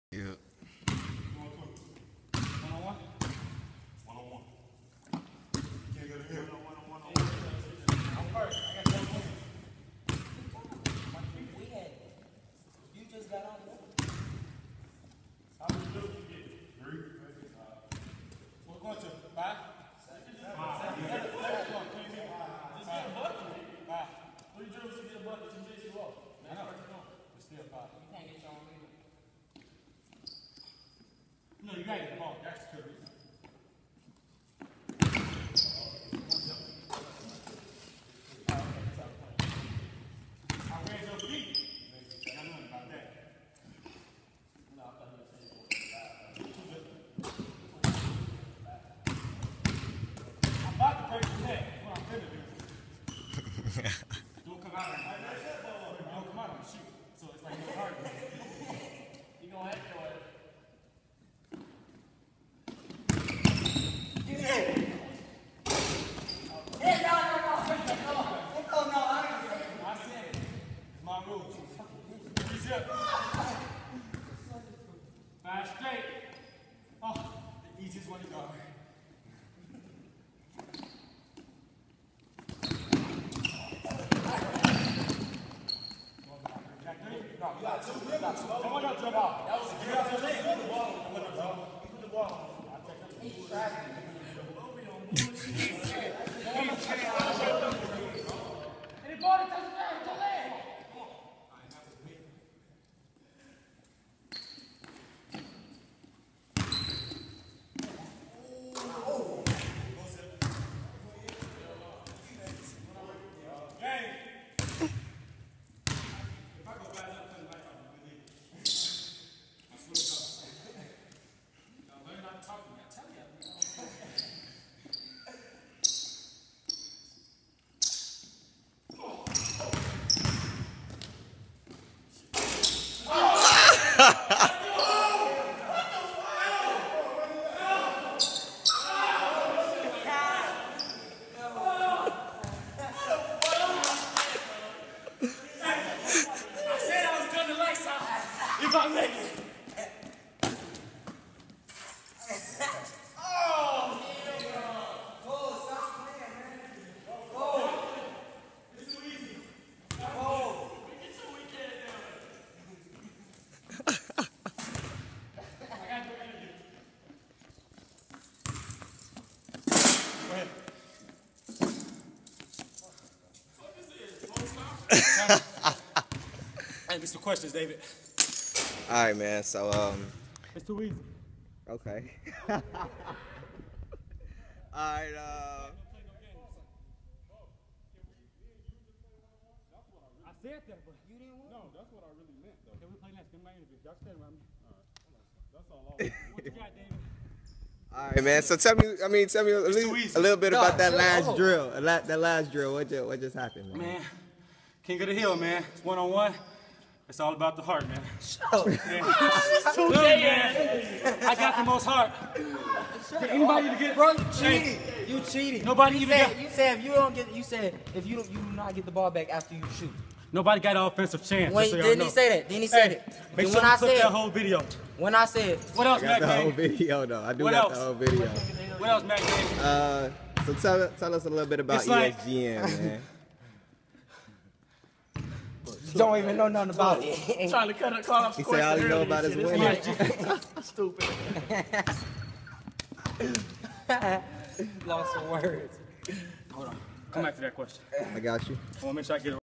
ESGN Training Camp: Coach Bo Gets Called Out
1on1 King Of The Hill ESGN Camp

Coach Bo Patton
BJ
Jeremiah Jones
Zarrius Stevens

ESGN: Elite Skills Got Next